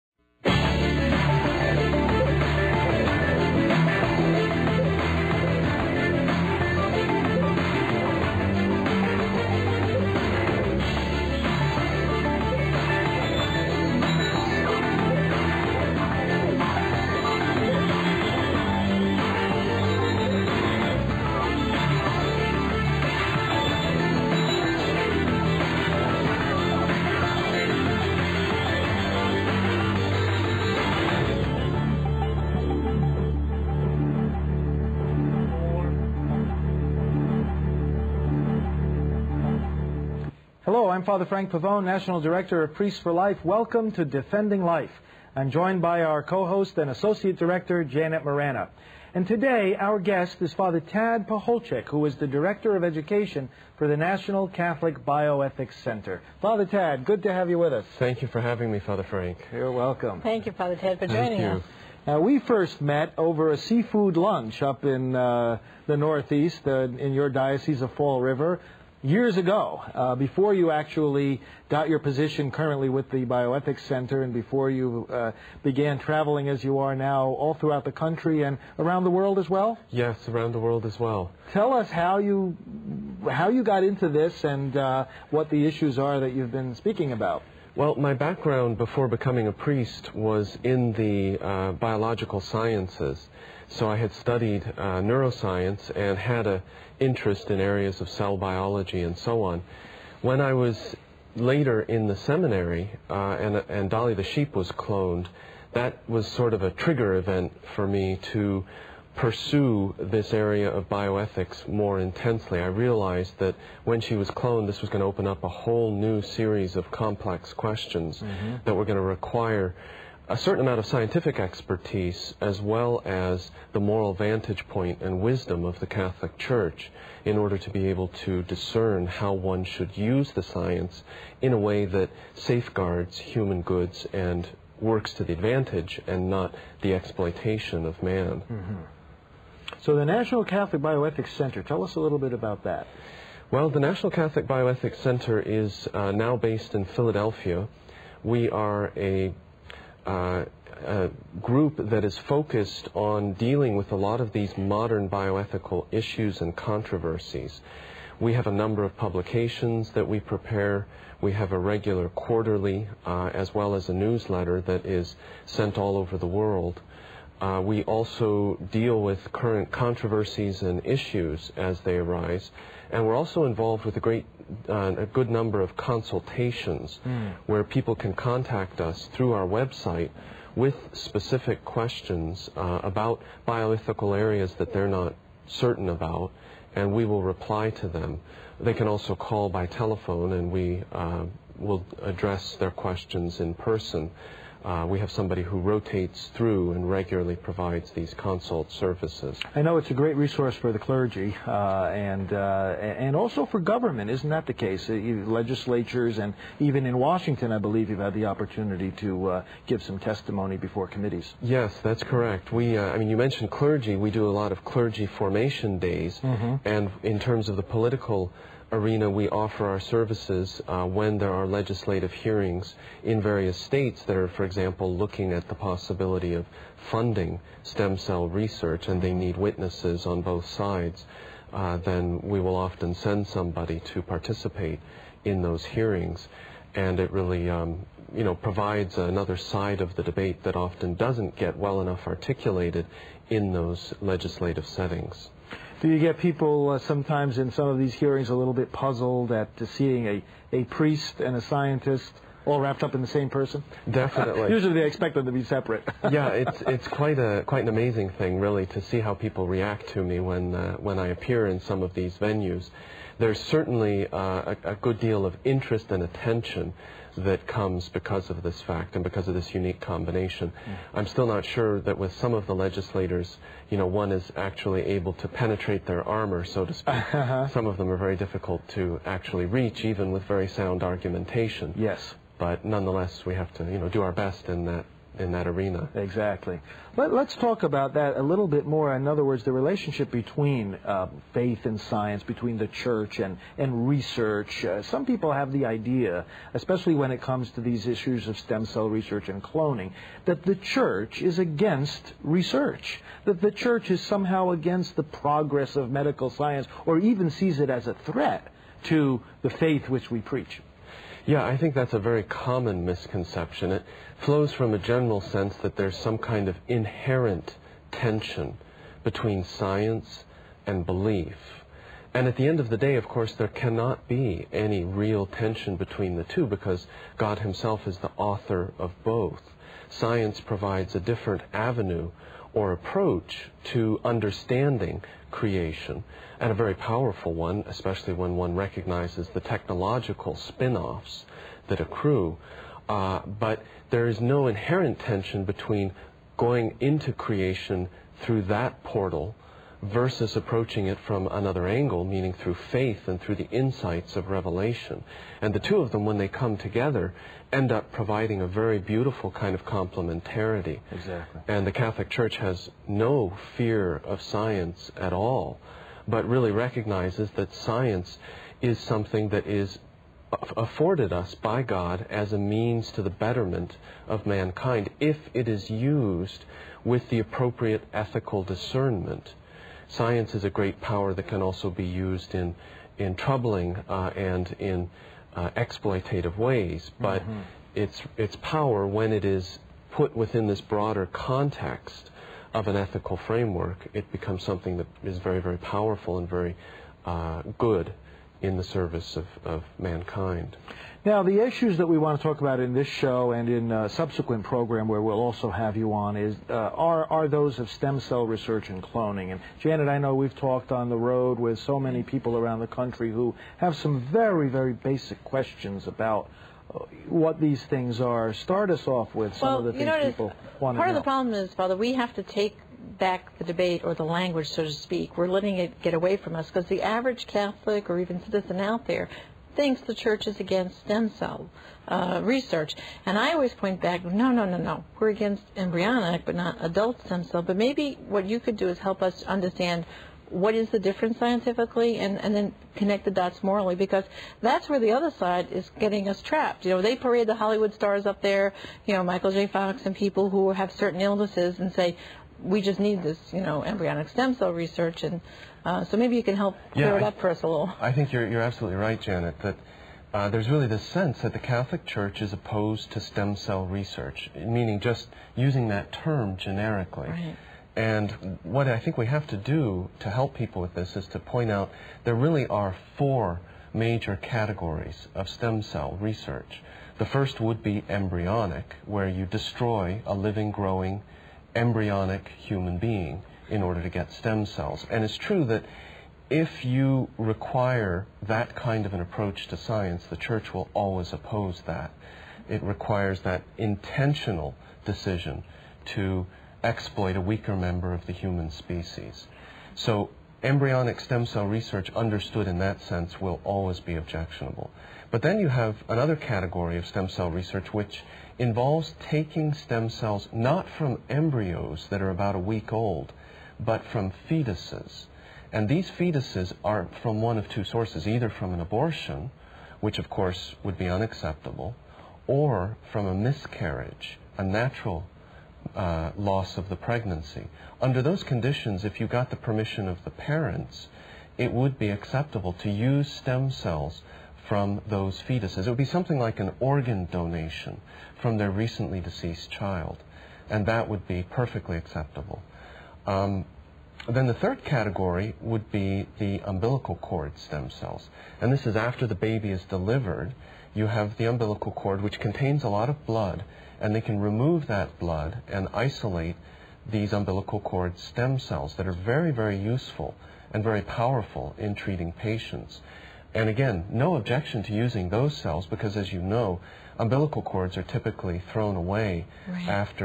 What Is Stem Cell Research?
Fr. Pavone and an expert bioethicist discuss the effectiveness and morality of the various types of stem cell research: embryonic, fetal, umbilical and adult.